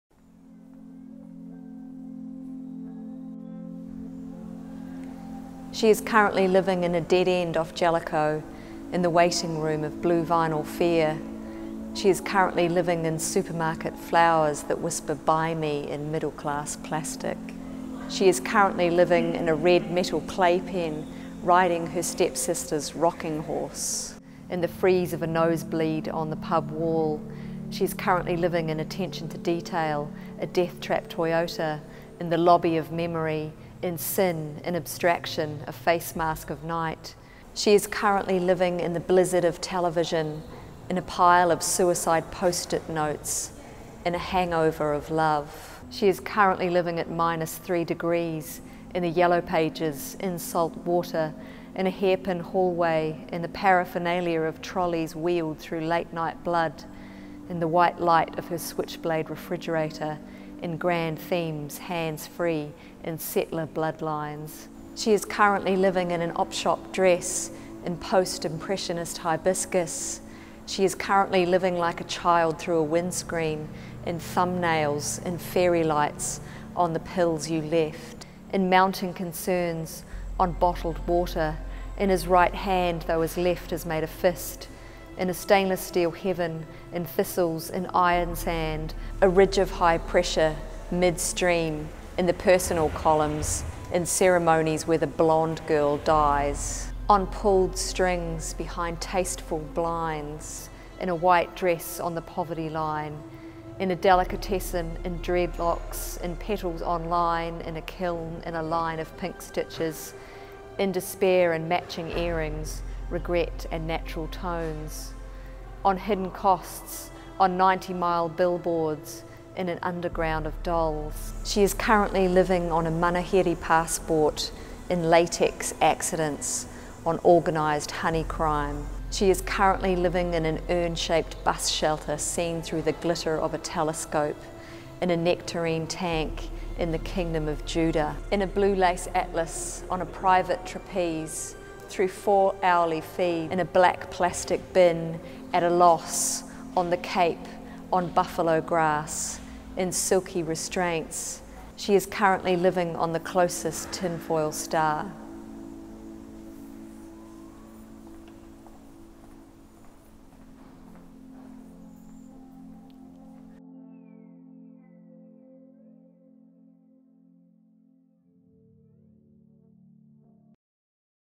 World Poetry Day - Dr Tracey Slaughter
"She is currently living in supermarket flowers that whisper buy me in their middle-class plastic..."

In celebration of World Poetry Day, we caught up with award-winning poet and University of Waikato Writing Studies lecturer, Dr Tracey Slaughter. She shared a reading of her beautiful poem 'She is currently living' with us.

Tracey was awarded second place winner in this year's Peter Porter Poetry Prize for her poem 'breather'.